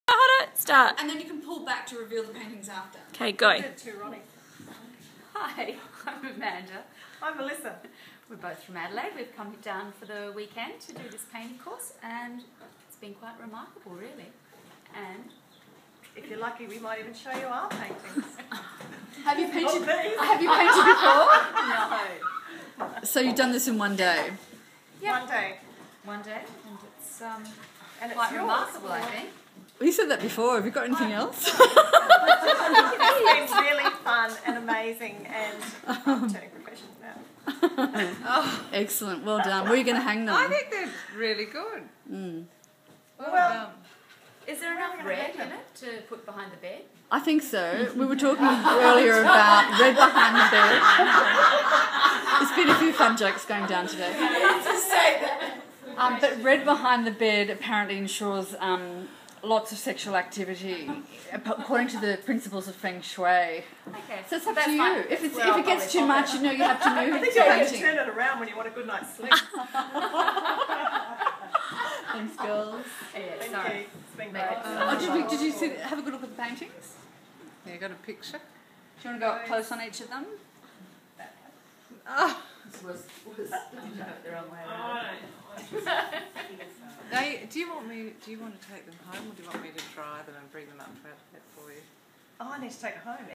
Masterpieces in a day
In January I was invited to give a One Day Blooms summer season workshop in Robe at Jude and Tim Rogers Gallery  26 new workshop space. Here is what two of the fabulous women produced. I am a fan of Robe and Gallery 26. Such a gorgeous place to visit.
The cool sea breezes were very welcome.